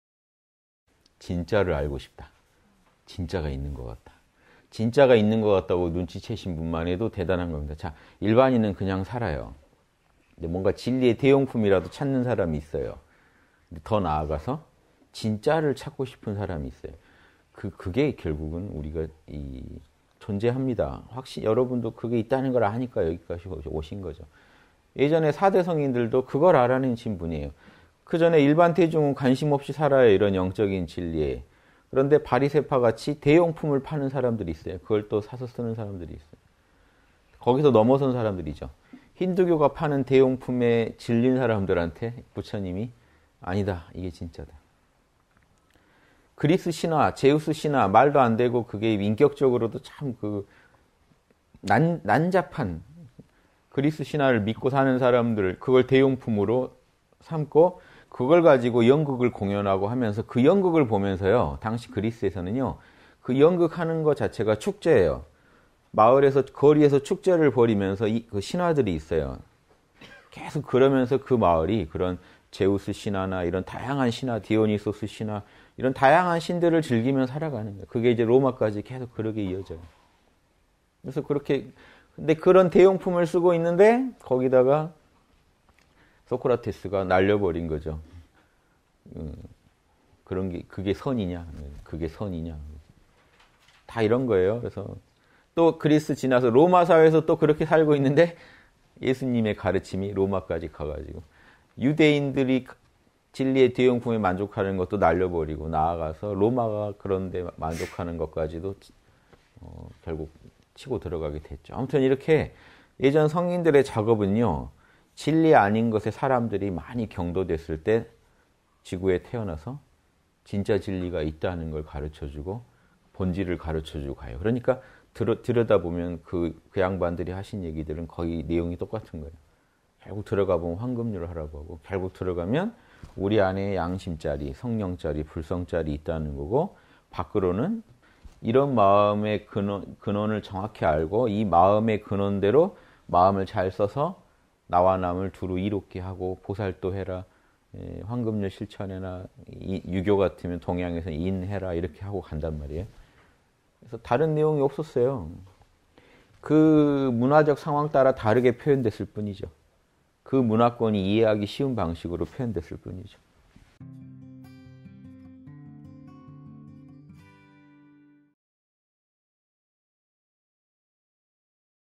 [3분 인문학] 진짜를 찾고 싶다_홍익선원.윤홍식
(170528 일요대담) ‘진짜를 알고 싶다.’
‘진짜가 있는 것 같다.’
진짜가 있는 것 같다고 눈치 채신 분만 해도 대단한 겁니다.
자, 일반인은 그냥 살아요.

(반면)뭔가 진리의 대용품이라도 찾는 사람이 있어요.
더 나아가서 진짜를 찾고 싶은 사람이 있어요.

여러분도 그게 있다는 걸 아니까 여기까지 오신 거죠.
예전에 사대 성인들도 그걸 알아내신 분이에요.

그전에 일반 대중은 관심 없이 살아요. 이런 영적인 진리에
근데 바리새파같이 대용품을 파는 사람들이 있어요.
그걸 또 사서 쓰는 사람들이 있어요.
거기서 넘어선 사람들이죠.

힌두교가 파는 대용품에 질린 사람들한테 부처님이
“아니다, 이게 진짜다”
그리스 신화, 제우스 신화, 말도 안 되고
그게 인격적으로도 참 그
난잡한, 그리스 신화를 믿고 사는 사람들
그걸 대용품으로 삼고 그걸 가지고
연극을 공연하고 하면서, 그 연극을 보면서요.

당시 그리스에서는요.
그 연극 하는 것 자체가 축제에요. 마을에서
거리에서 축제를 벌이면서 신화들이 있어요.
계속 그러면서 그 마을이 그런 제우스 신화나 이런 다양한 신화들,
디오니소스 신화
이런 다양한 신들을 즐기면서 살아가는 거에요.

그게 이제 로마까지 계속 그렇게 이어져요.
그래서 그렇게 그런 대용품을 쓰고 있는데 거기다가
소크라테스가 날려버린 거죠. “그런 게 선이냐?”
다 이런 거에요.

그래서 또 그리스 지나서 로마사회에서도
또 그렇게 살고 있는데
예수님의 가르침이 로마까지 가서 유대인들이
진리에 대용품을 만족하는 것도 날려버리고
나아가서 로마 그런 데 만족하는 것 까지도
결국 치고 들어가게 됐죠.

아무튼 이렇게 예전 성인들의 작업은요. 진리 아닌 것에
사람들이 많이 경도됐을 때,
지구에 태어나서 진짜 진리가 있다는 걸 가르쳐 주고,
본질을 가르쳐 주고 가요.

그러니까 들여다보면 그 양반들이 하신 얘기들은
거의 내용이 똑같은 거에요.

결국 들어가 보면 황금률을 하라고 하고,
결국 들어가면 우리 안에 양심자리,
성령자리, 불성 자리 있다는 거고 밖으로는
이런 마음에 근원을 정확히 알고
이 마음에 근원대로 마음을 잘 써서 나와 남을
두루 이롭게 하고, 보살도 해라,
황금률 실천해라 동양의 유교 같으면 인(仁)해라 이렇게 하고
간단 말이에요. 그래서 다른 내용이 없었어요.

그 문화적 상황 따라 다르게 표현됐을 뿐이죠.
그 문화권이 이해하기 쉬운 방식으로 표현됐을 뿐이죠.